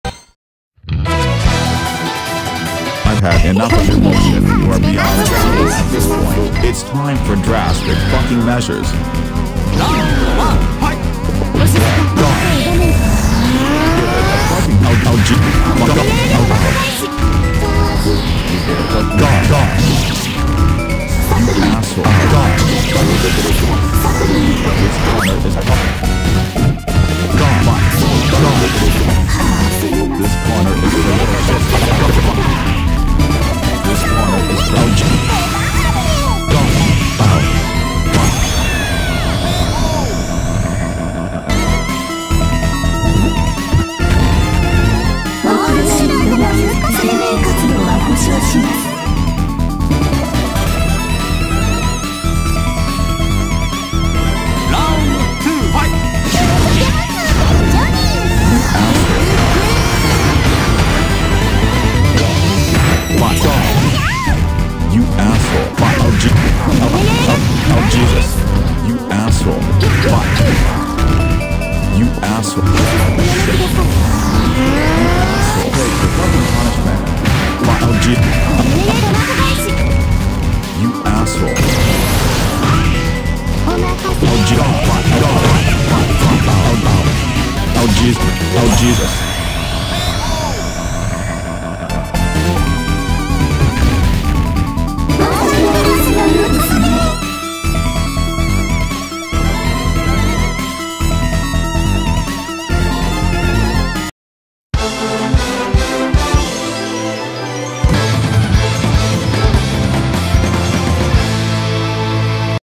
Hisui, Kohaku, Mech Hisui, and the fleet (Me) vs Dawn De Era Boris Anderson
The Maids punish Anderson for his abuse of his children!  Enjoy!